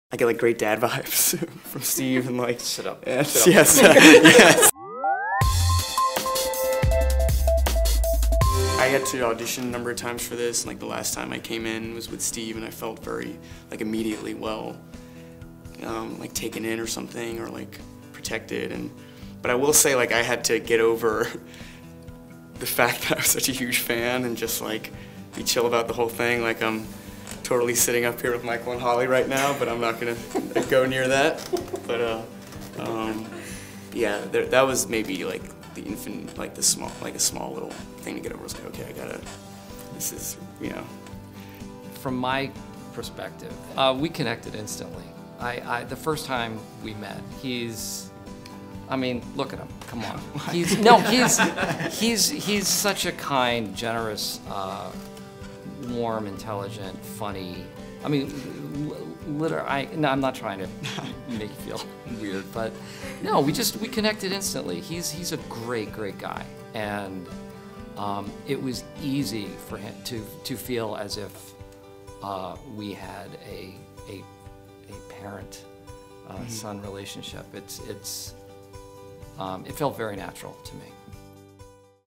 Timothee Chalamet Freaked Out About Working With Steve Carell & Amy Ryan | TIFF 2018 | PeopleTV
The 'Beautiful Boy' co-stars on working together and their immediate connection.